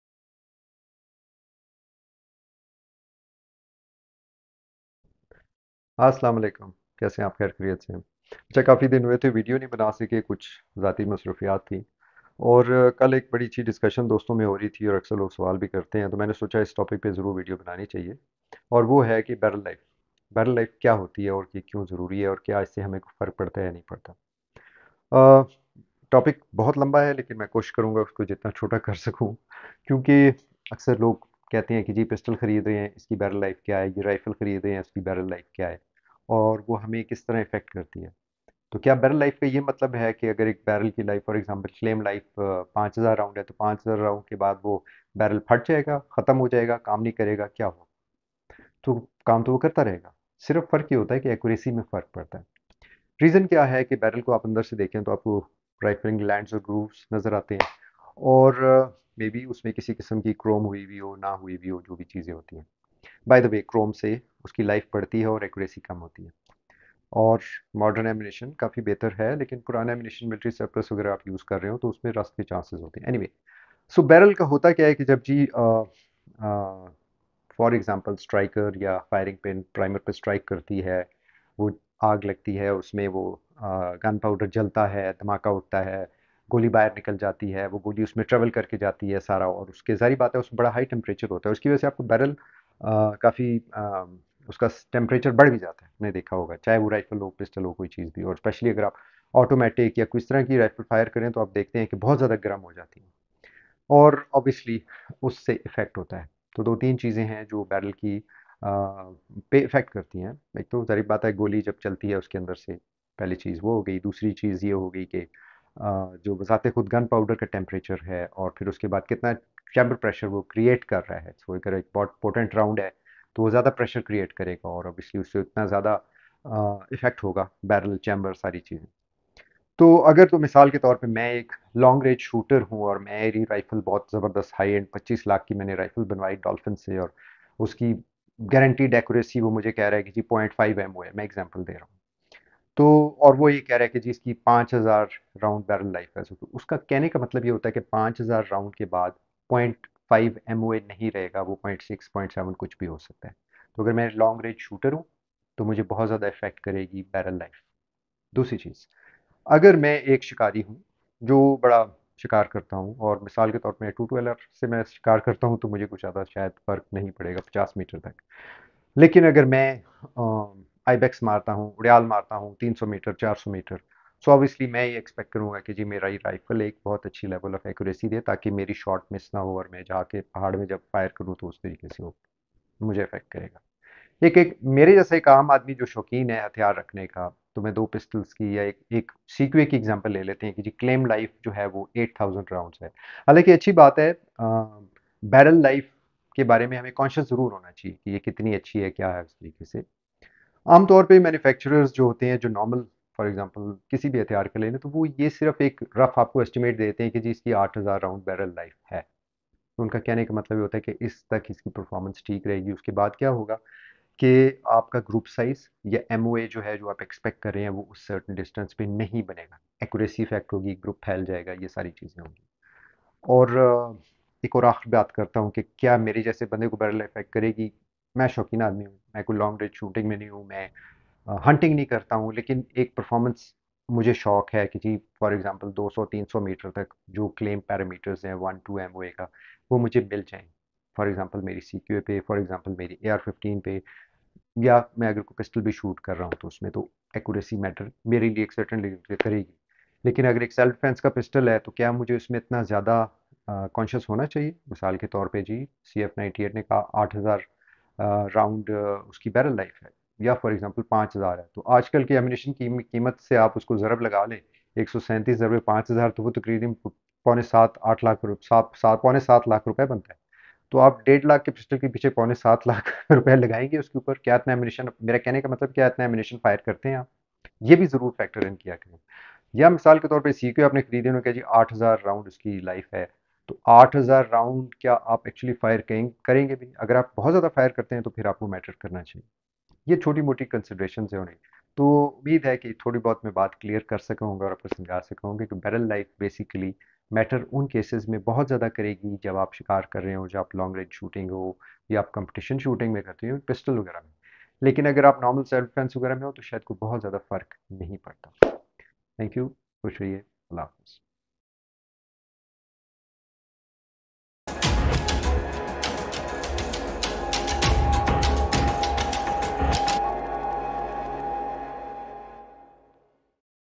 What is barrel life? And does it matter?
what is barrel life? does it matter? Is barrel life important? what factors reduce barrel life?